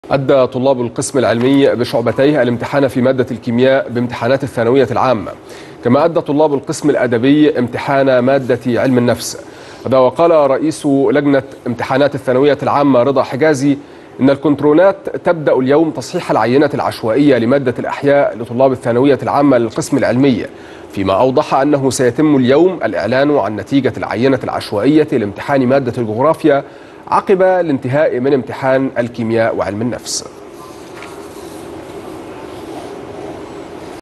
بدء تصحيح العينة العشوائية من مادة الأحياء وإعلان نتيجة العينة العشوائية لمادة الجغرافيا
الموقع الرسمي للقناة  ..